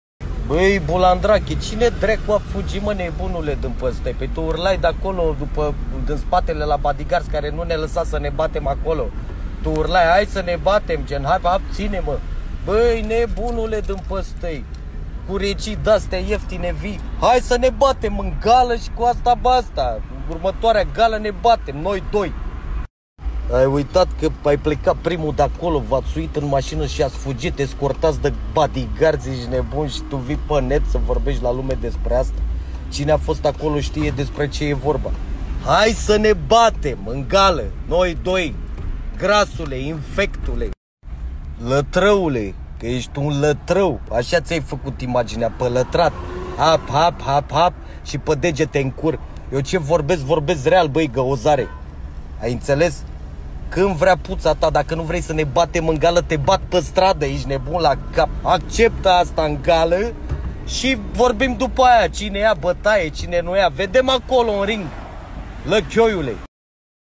Necunoscutul de Jon Zdreanta spune minciuni despre ce s-a întâmplat la gală
Cheliosule, numai face tu pe deșteptul, că tu erai protejat de brigada lui Zvelea și îți era frică să-l înfrunți